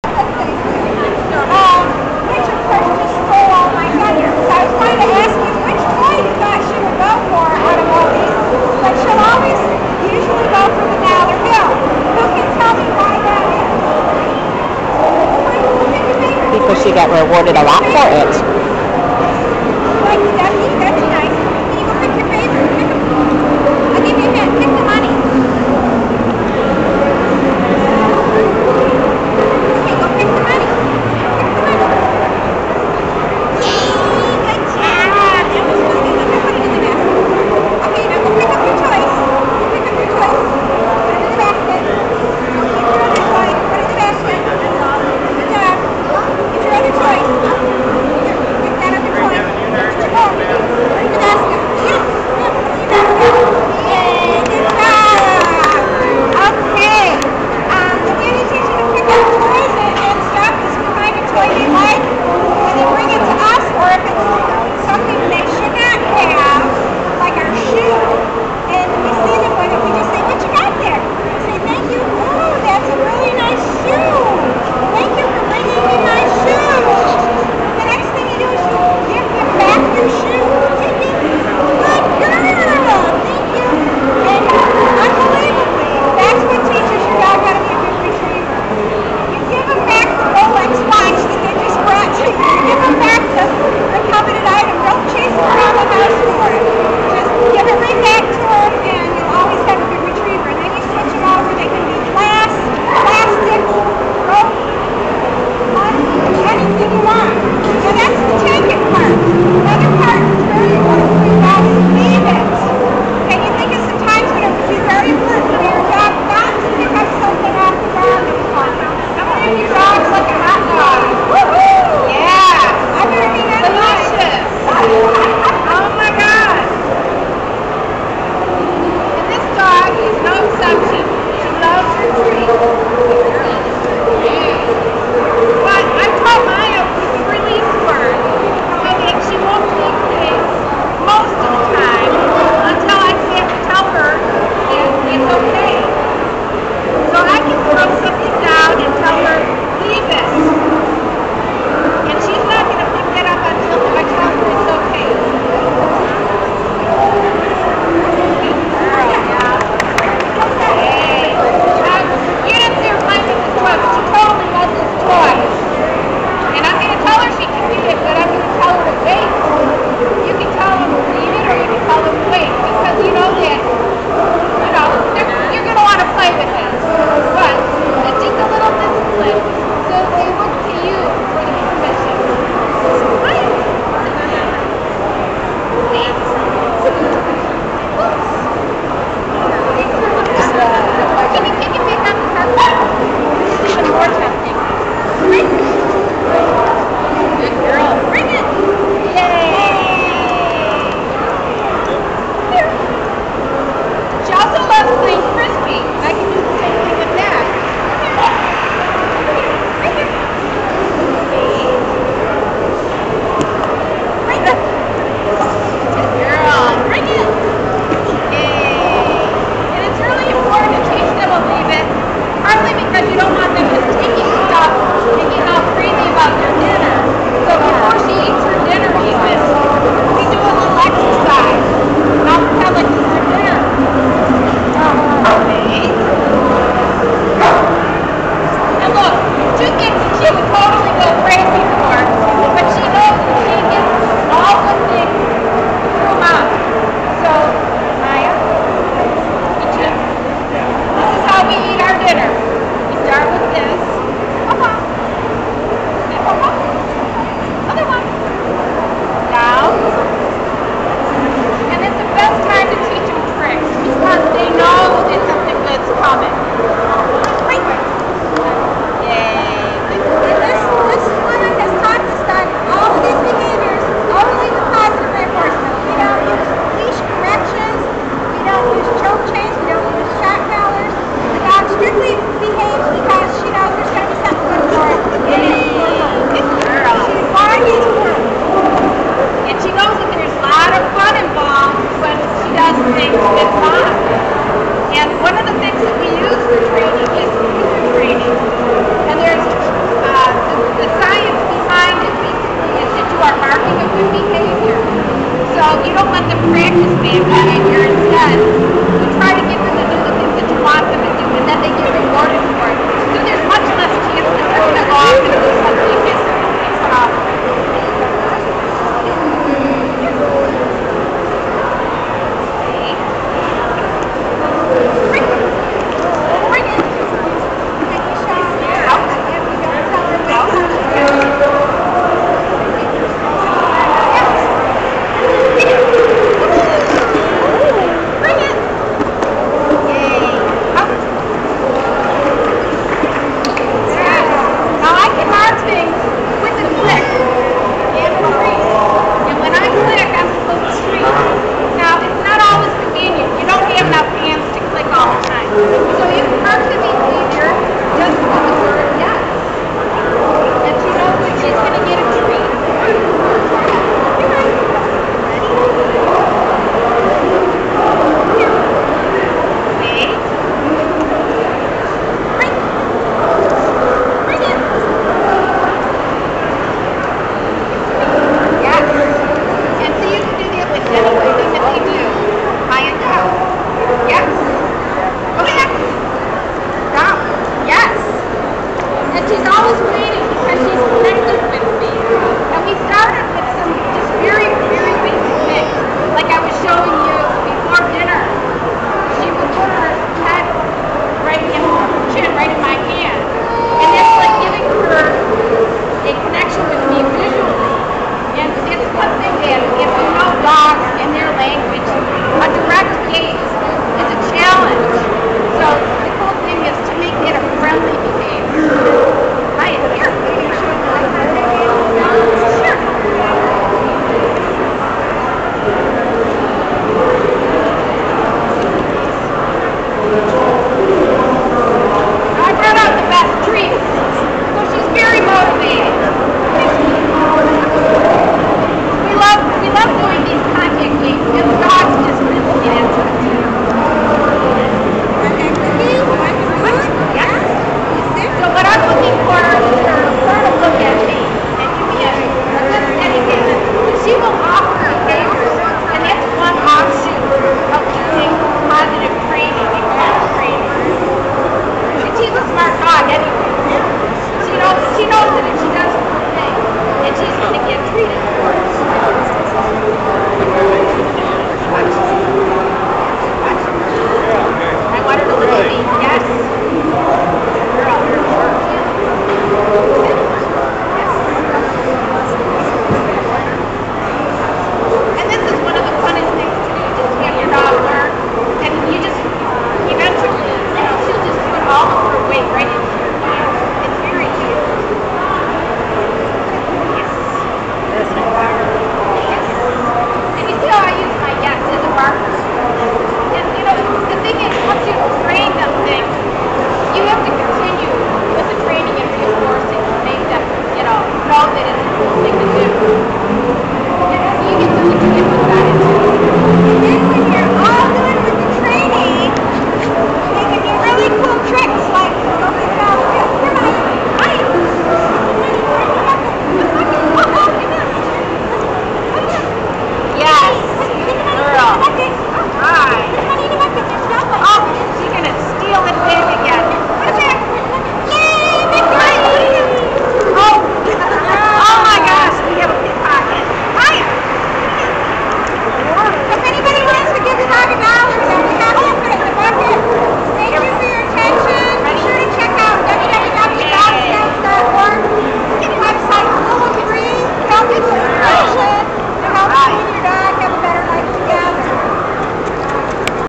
Dog Scout Demo at Cobo Hall 2011
Lonnie, Kozi, Martha and Maia demonstrate some of the things you can teach your dog with positive training.  Demo was at the Dog Scouts of America booth at Cobo Hall, Detroit, Mi during the DKC Dog Show.